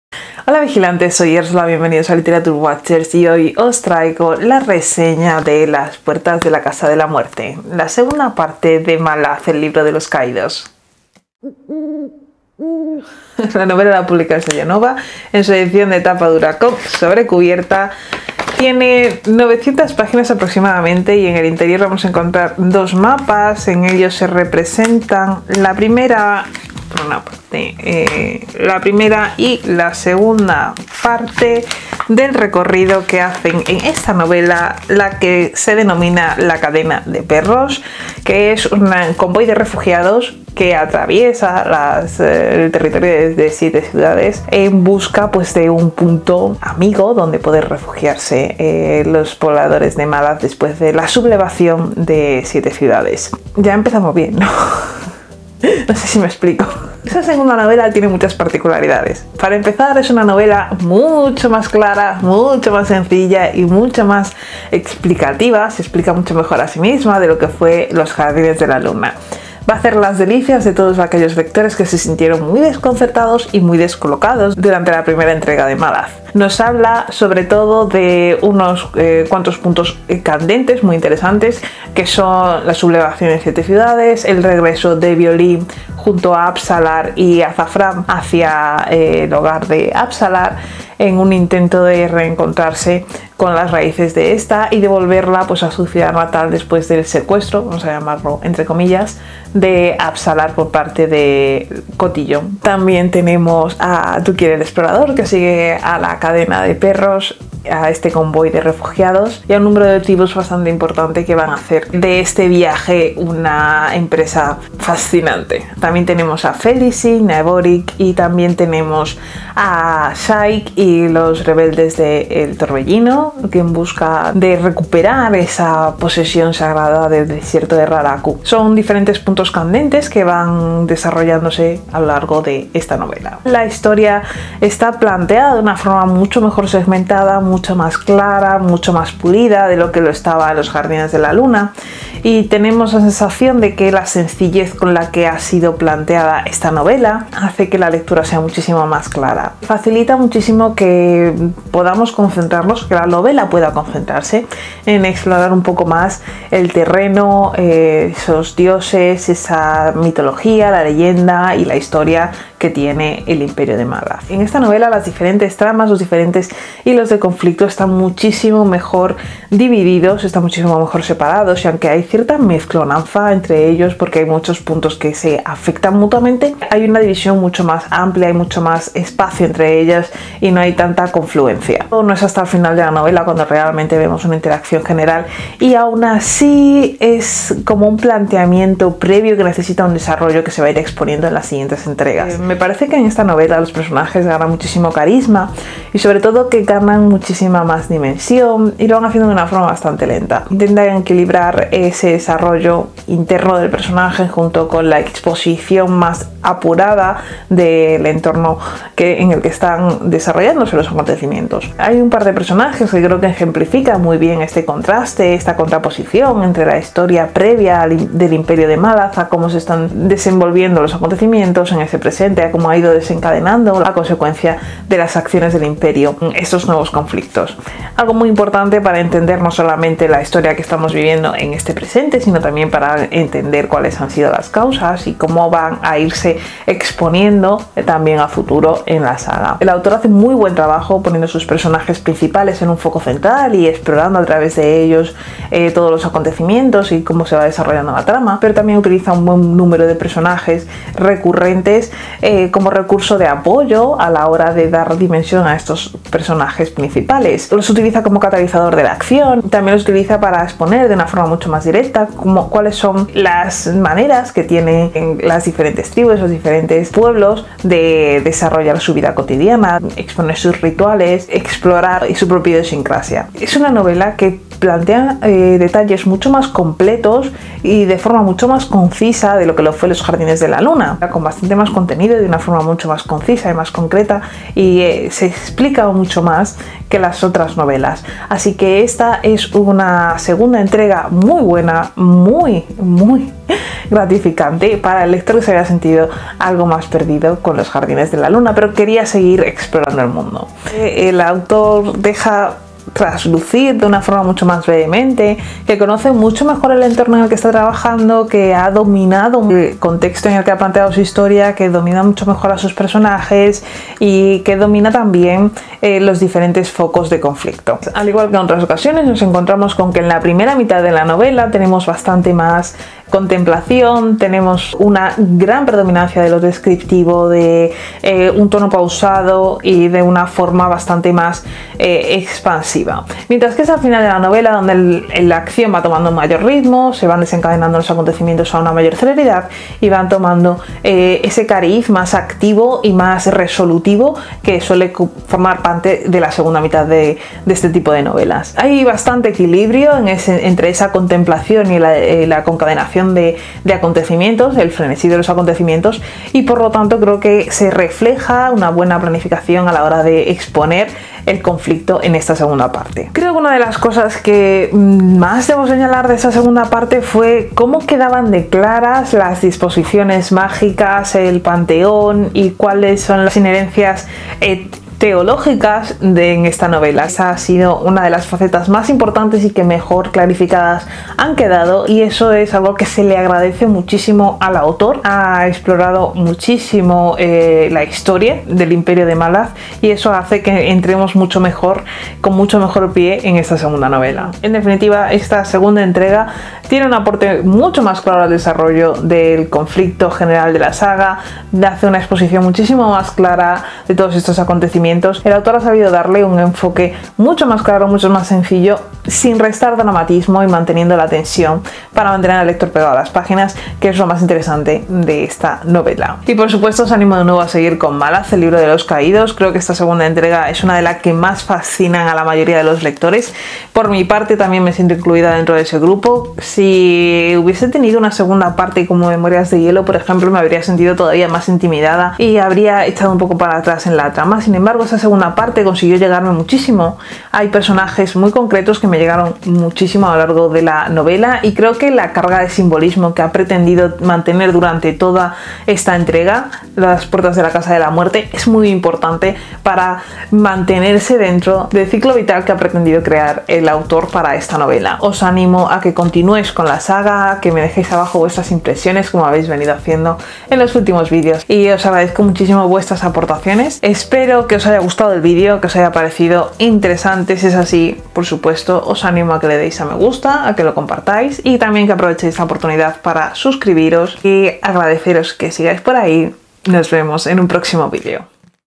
Reseña Malaz El libro de los caídos | Las puertas de la casa de la muerte Steven Erikson
Opinión de la segunda novela de la saga de Malaz. El libro de los caídos, Las puertas de la casa de la muerte.
Espero que os anime a continuar esta saga de fantasía épica.
Contadme también  vuestra opinión en los comentarios.

Las puertas de la casa de la muerte. Saga Malaz. El libro de los caídos de Steven Erikson